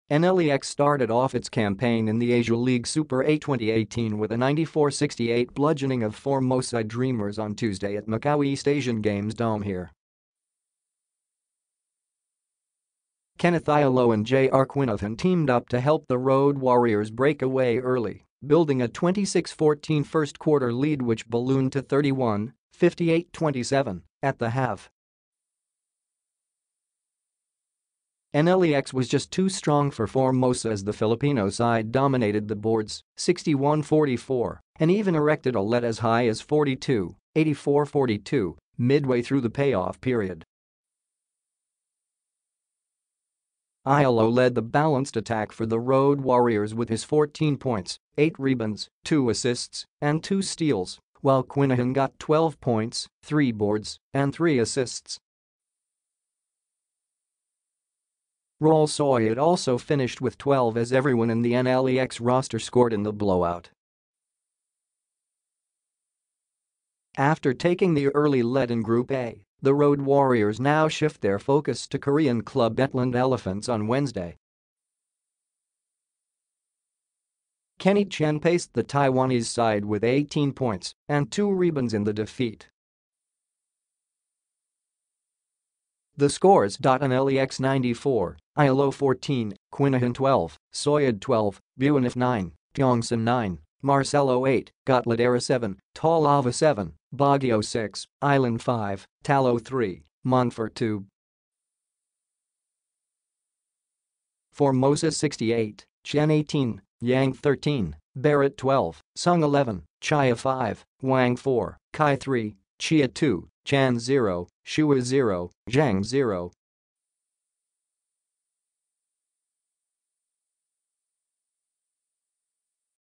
Ighalo seizes opportunity, leads NLEX rout of Dreamers in Macau
NLEX started off its campaign in the Asia League Super 8 2018 with a 94-68 bludgeoning of Formosa Dreamers on Tuesday at Macao East Asian Games Dome here.

Kenneth Ighalo and JR Quinahan teamed up to help the Road Warriors break away early, building a 26-14 first quarter lead which ballooned to 31, 58-27, at the half.

NLEX was just too strong for Formosa as the Filipino side dominated the boards, 61-44, and even erected a lead as high as 42, 84-42, midway through the payoff period.

Ighalo led the balanced attack for the Road Warriors with his 14 points, eight rebounds, two assists, and two steals, while Quinahan got 12 points, three boards, and three assists.

Raul Soyud also finished with 12 as everyone in the NLEX roster scored in the blowout.

After taking the early lead in Group A, the Road Warriors now shift their focus to Korean club ETLAND Elephants on Wednesday.

Kenny Chien paced the Taiwanese side with 18 points and two rebounds in the defeat.

The scores:

NLEX 94 – Ighalo 14, Quinahan 12, Soyud 12, Buenafe 9, Tiongson 9, Marcelo 8, Gotladera 7, Taulava 7, Baguio 6, Uyloan 5, Tallo 3, Monfort 2.

FORMOSA 68 – Chien 18, Yang 13, Barratt 12, Sung 11, Chieh 5, Wang 4, Chai 3, Chia 2, Chen 0, Hsueh 0, Zheng 0.